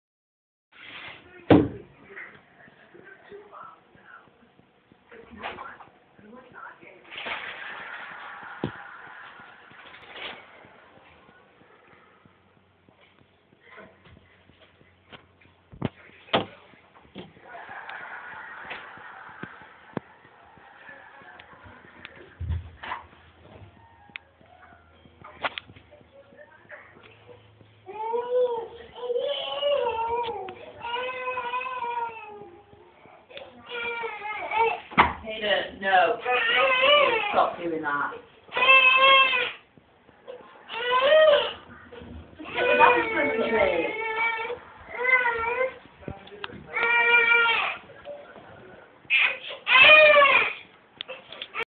Chinese water dragon
My 2 chinise water dragon. i would like to kno sex of them im sure its male n fmale